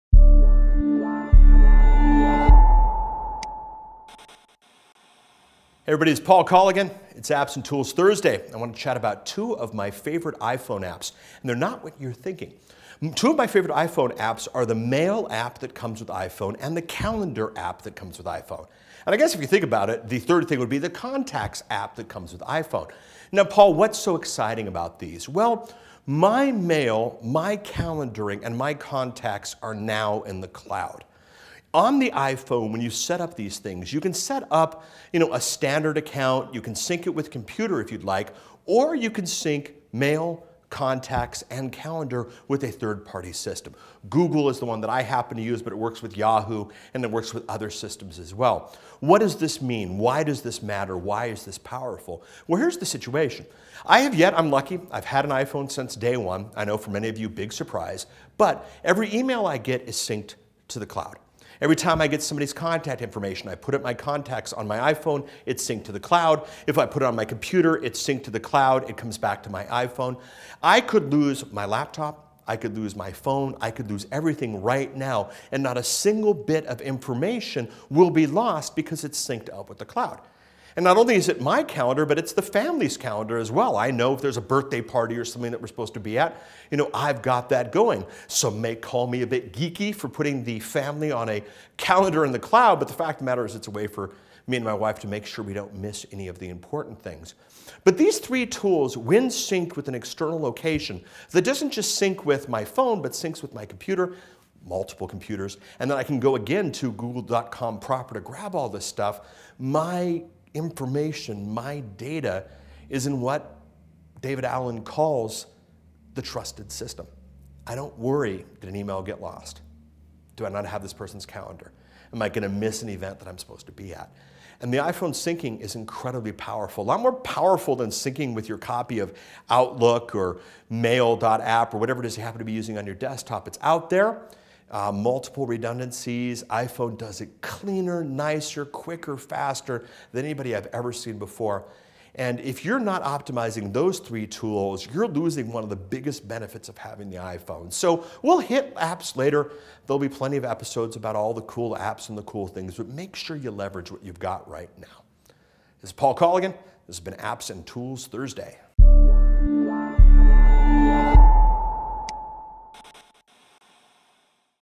Mail & Calendar App for iPhone/iPad - AppsNToolsThursday
The mail and calendar app for the iPhone/iPad are two of Paul Colligan's favorite apps. Find out how these apps can be synced with a third party system such as Google.